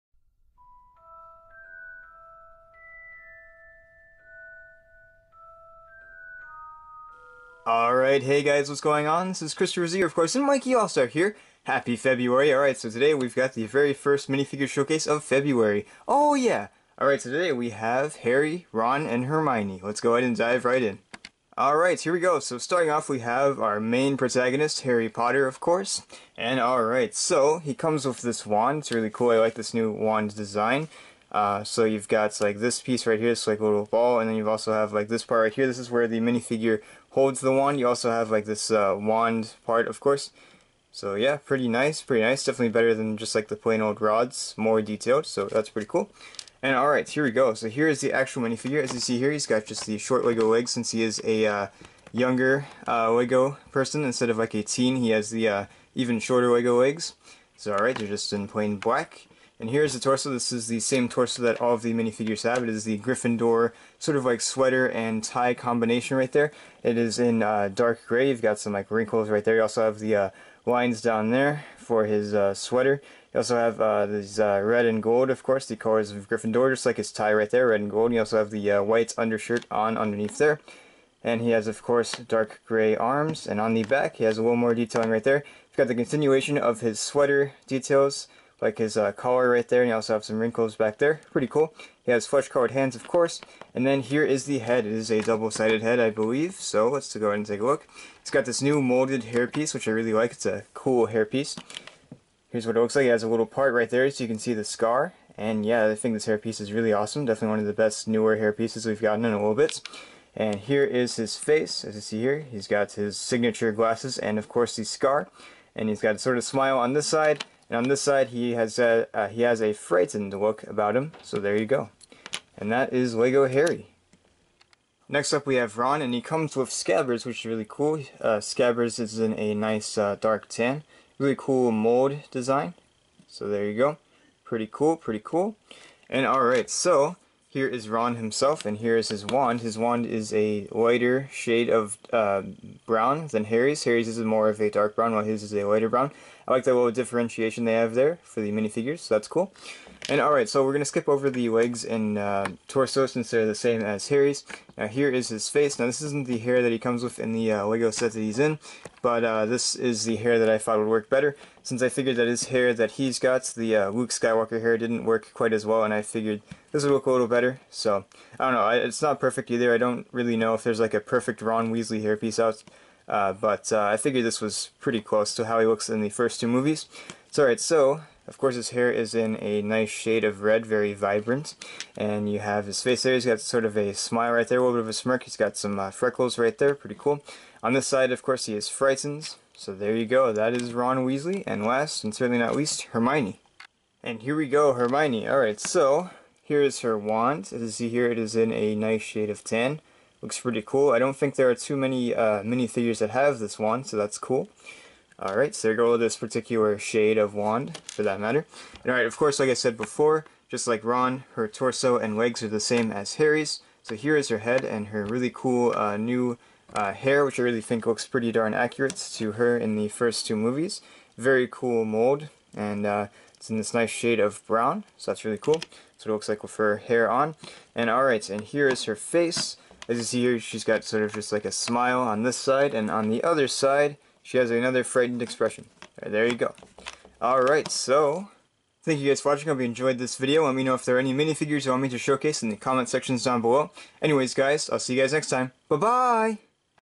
Minifigure Showcase: Harry, Ron, and Hermione
Welcome back to Minifigure Showcase! Today, we're taking a look at Lego Harry, Ron, and Hermione from Lego Harry Potter (2018). Enjoy!!

Sorry, meant to post this last week.